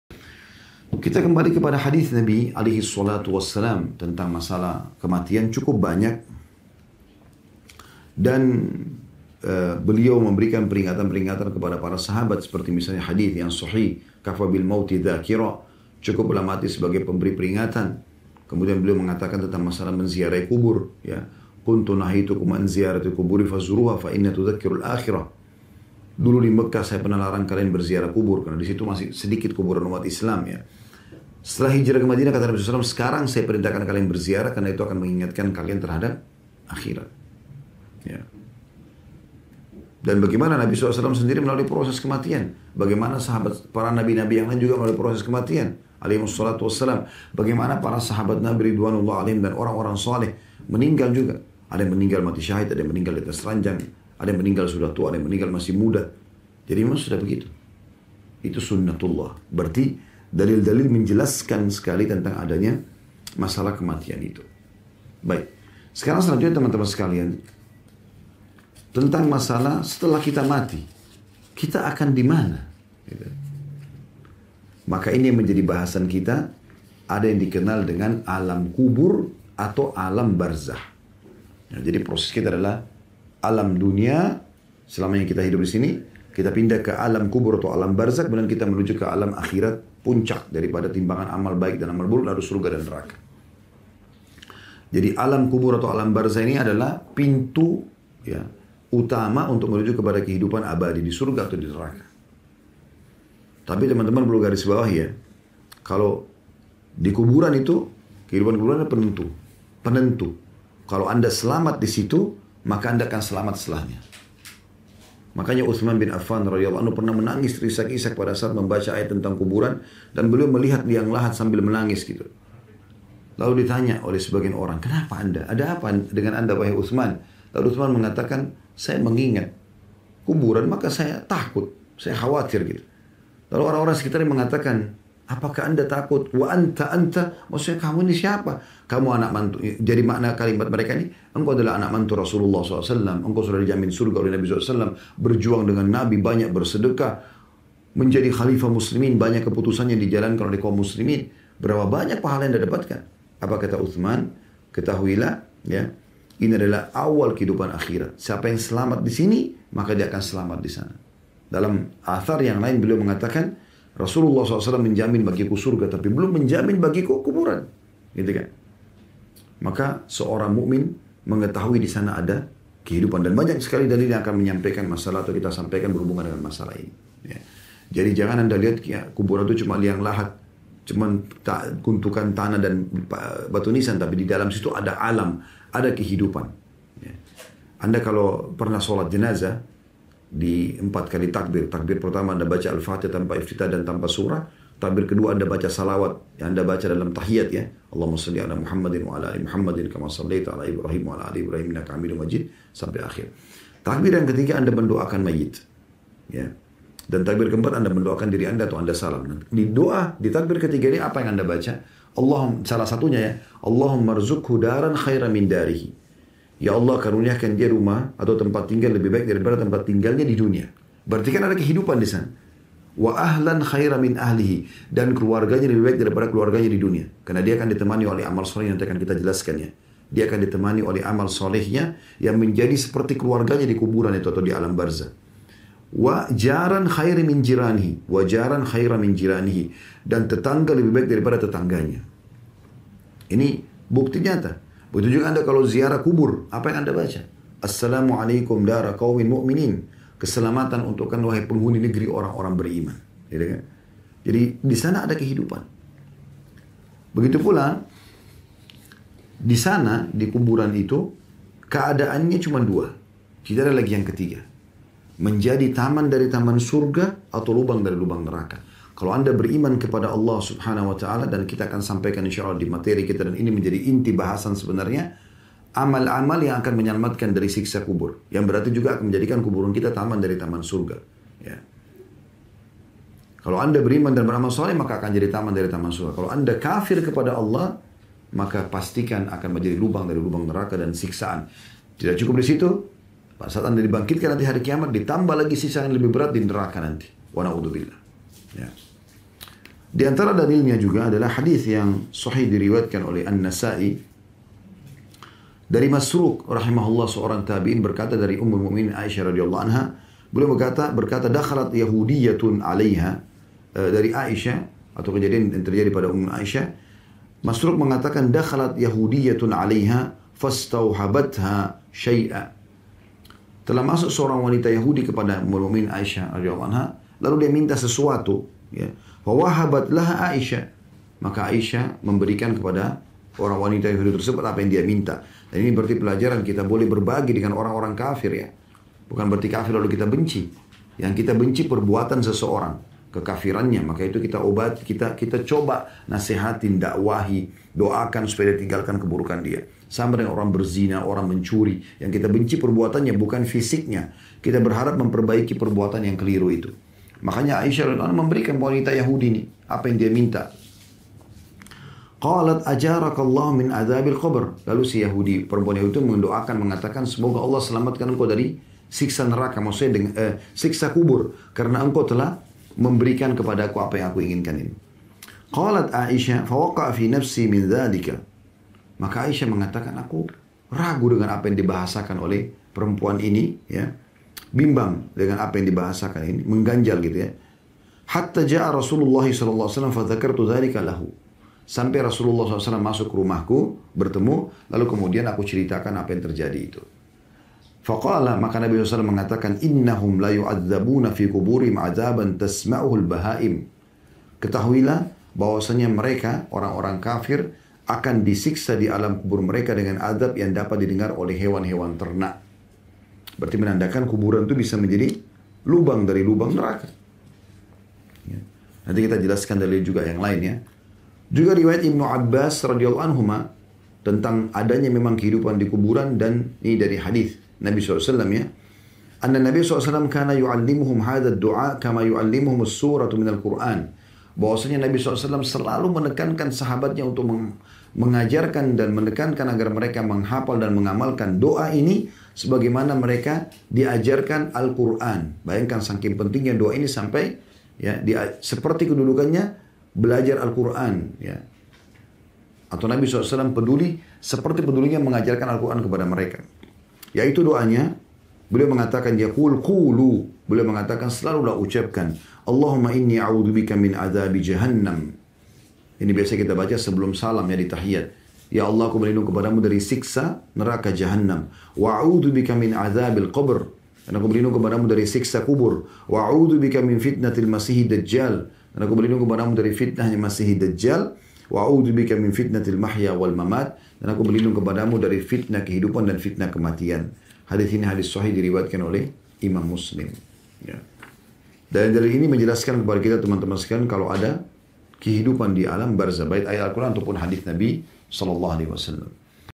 Setelah Kita M4TI Kita Akan Dimana ? Kajian Ustadz Khalid Basalamah
Nabi Muhammad Shallallahu alaihi wa sallam bersabda:
بلغوا عني ولو آية
Artinya: "Sampaikanlah dariku walau hanya satu ayat." (HR Bukhari)